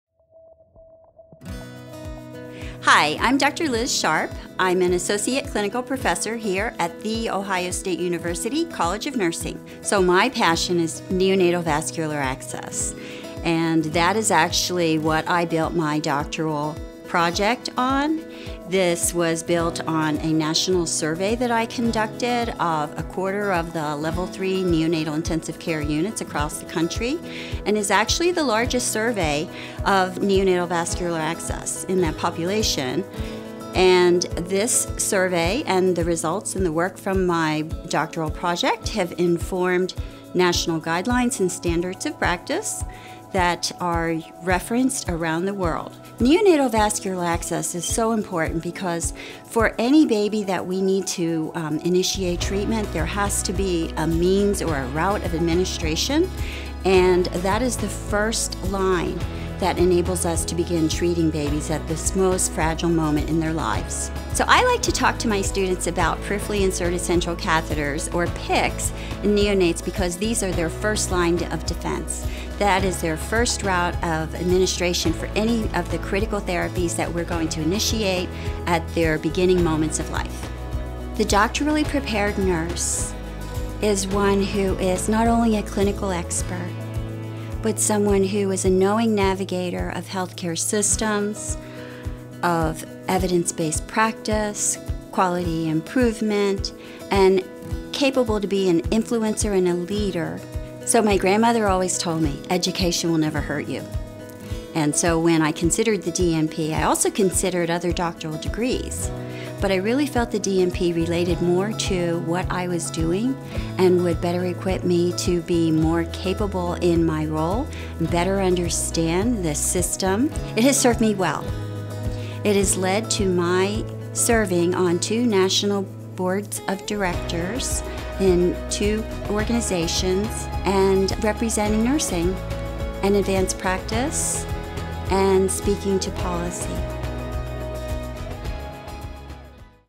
DNP Faculty Spotlight - Elizabeth Sharpe
Associate Clinical Professor Elizabeth Sharpe, DNP, APRN-CNP, FNAP, FAANP, FAAN, discusses her passion for neonatal care, helping babies at very fragile points of their lives and engaging her students in answering their calling to care. She also explains why the Doctor of Nursing Practice (DNP) degree she earned has been so critical for her career.